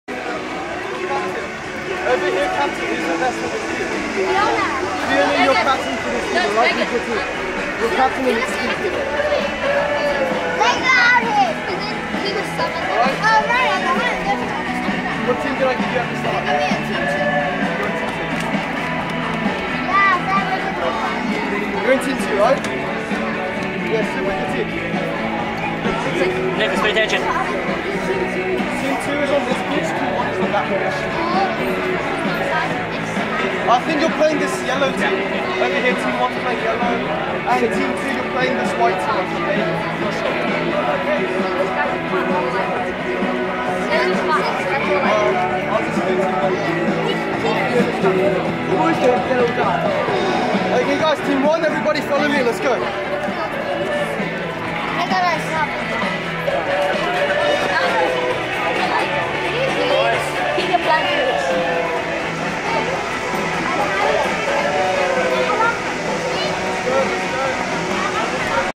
Primary Touch Rugby Showcase in King's Park, Hong Kong - 2
Briefing before the game